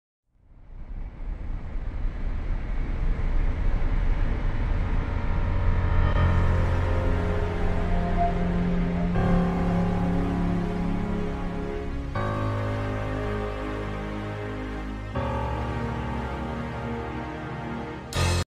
Los ultimos segundos de DART impactando Dimorphos 😱😱😱😱😱😱
Este tremendo video se ven los ultimos segundos antes del impacto de DART contra la Luna de Didymos. Son los 19 segundos mas emocionantes del año! 😱😱😱😱😱😱😱😱😱😱😱😱

La ultima imagen se ve una parte roja porque ya choco y no llego a transmitirla completa.

En las ultimas imagenes, las rocas tienen centimetros apenas.